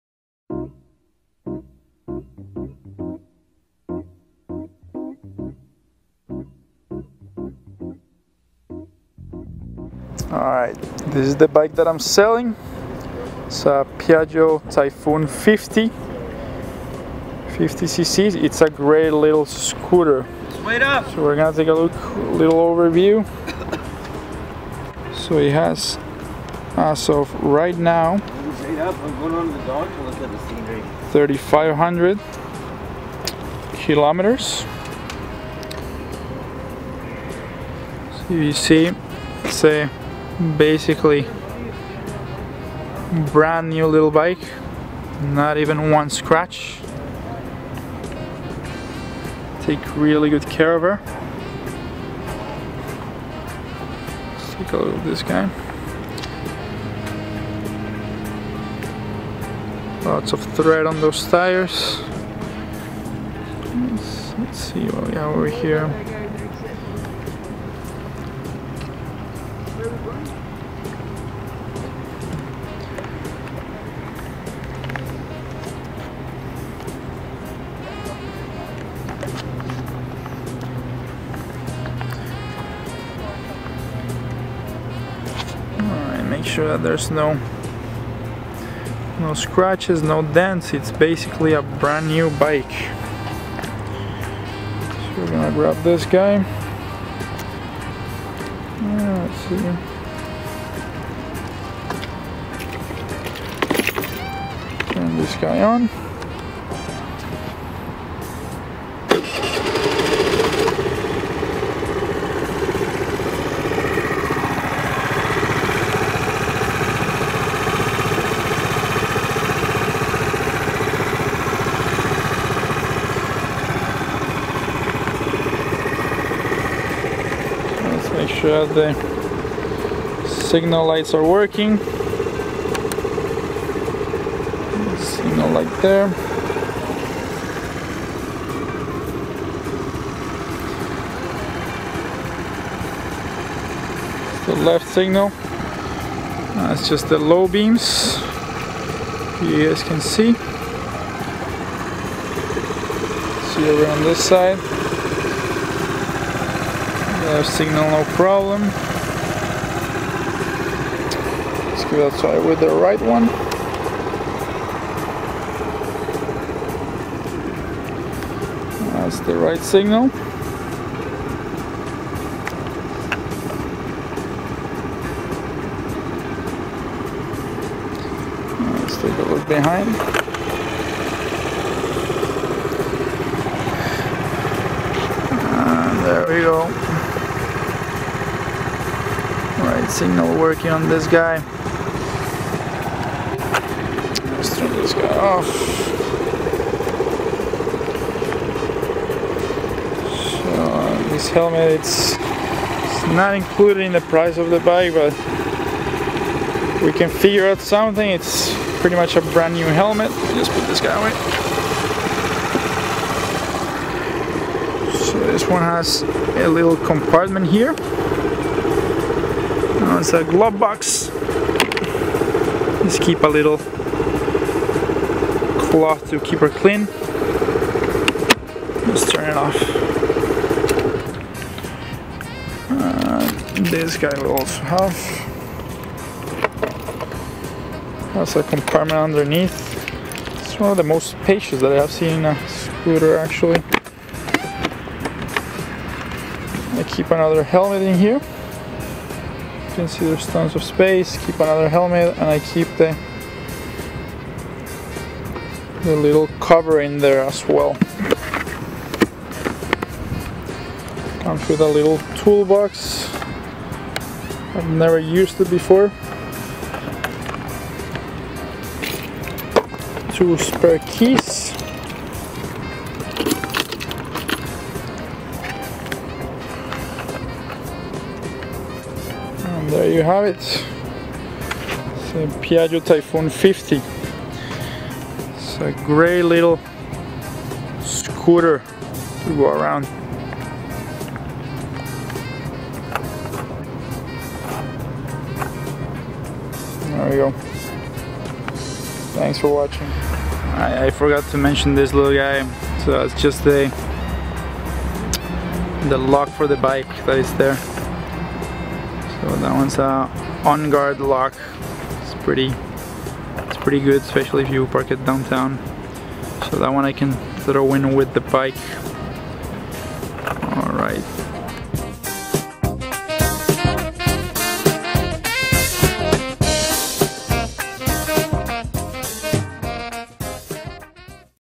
2013 Piaggio Typhoon 50cc - For Sale
For Sale - Piaggio Typhoon 50cc - Like New Condition
Video taken in Lower Lonsdale North Vancouver